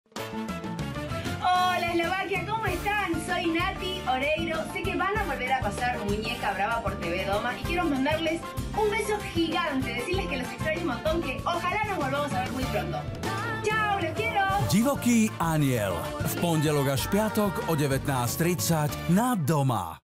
Divoký anjel - exkluzívny pozdrav od Natalie Oreiro
Natalia exkluzívne pre divákov televízie Doma nahrala pozdrav, ktorý musí rozhodne vidieť každý fanúšik Divokého anjela. Nabije vás energiou na nový pracovný týždeň. Natalia je skrátka živel, a preto ju milujeme! Viac o seriáli